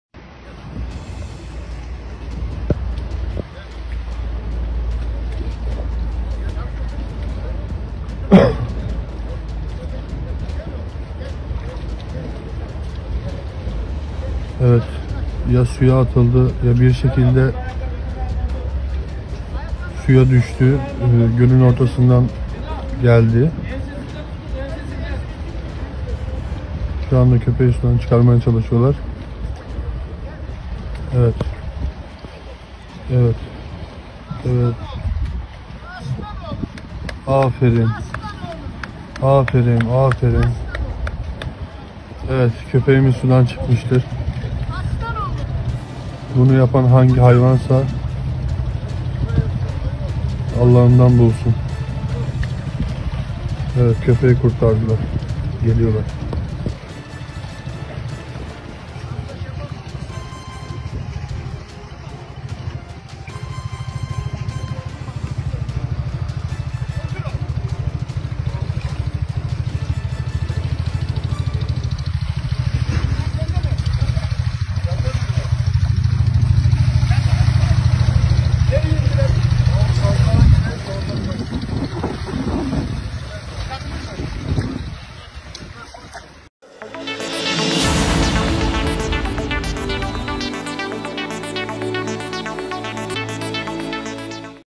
Gölde mahsur kalan köpeği böyle kurtardılar
Sapanca Gölü açıklarında yorgunluktan ve soğuktan donmak üzere olan kurt köpeği, amatör balıkçılar tarafından fark edilerek karaya çıkartıldı.